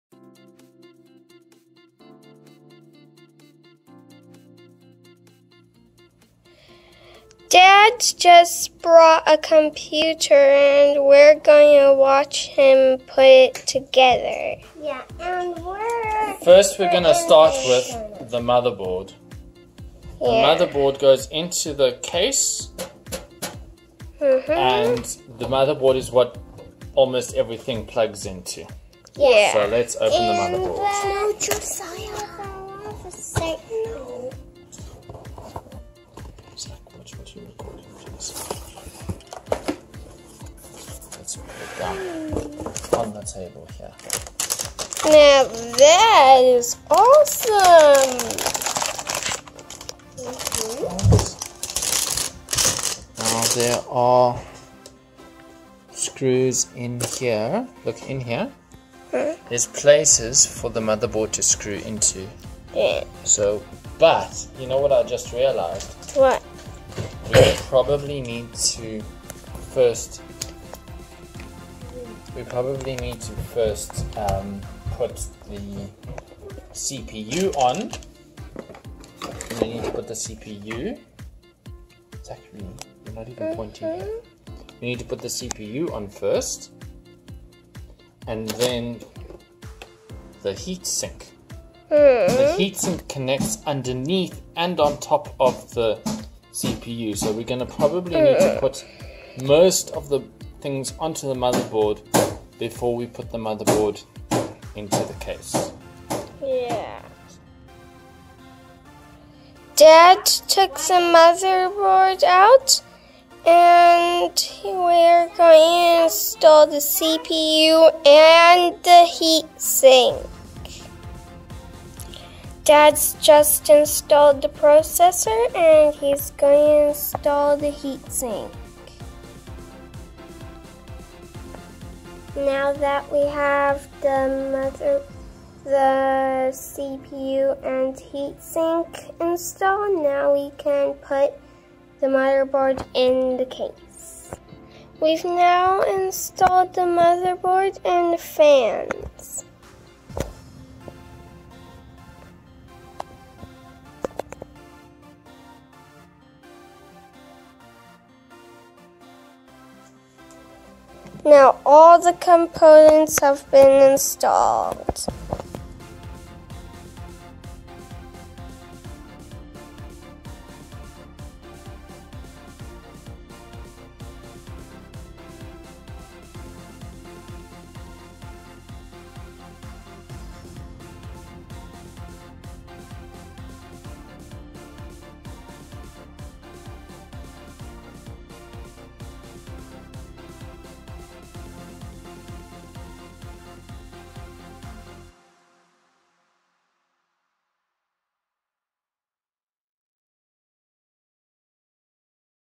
Assembling Dad's PC (a video by Zak)
Zak records Dad building his new computer.